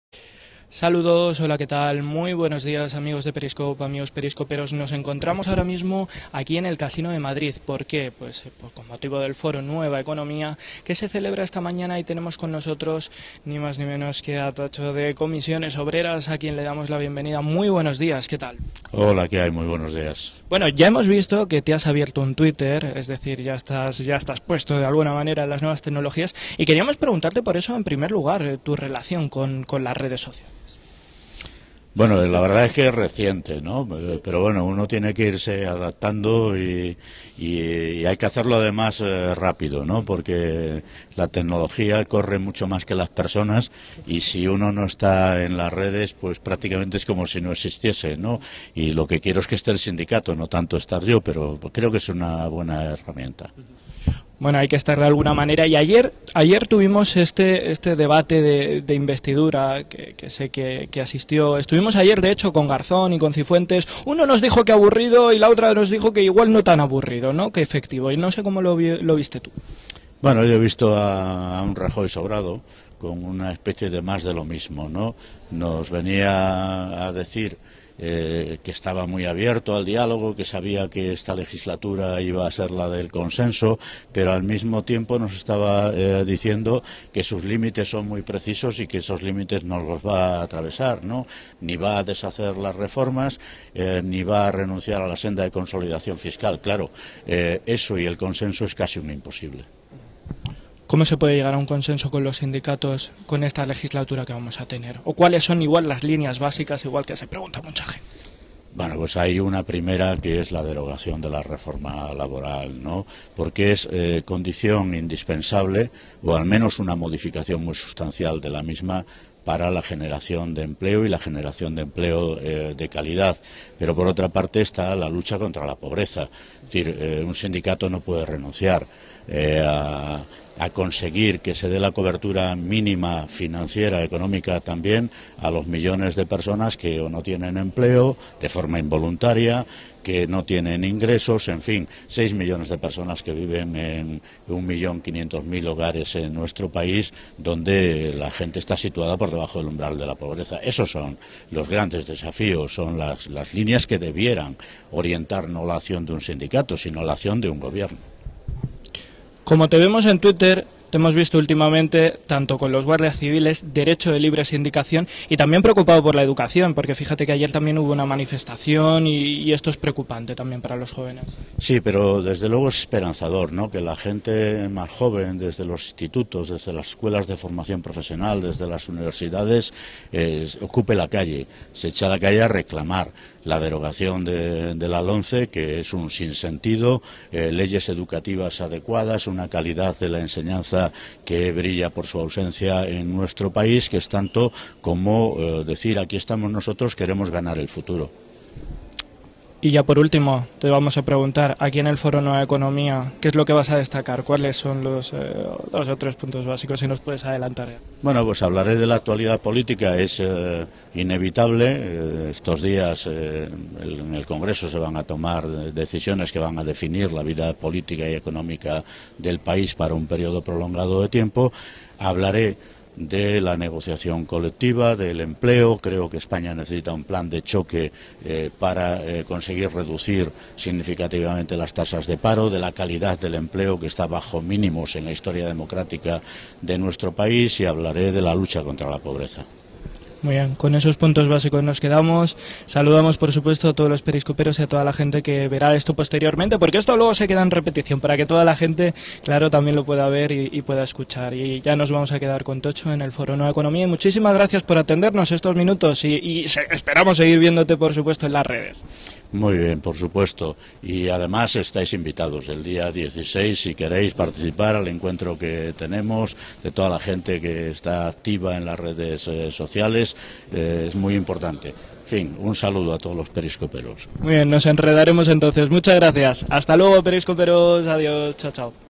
Ignacio Fernandez Toxo con Periscoperos en el Foro de Nueva Economia
Periscoperos entrevistan a Ignacio Fernandez Toxo antes de su intervención en el Foro Nueva Economía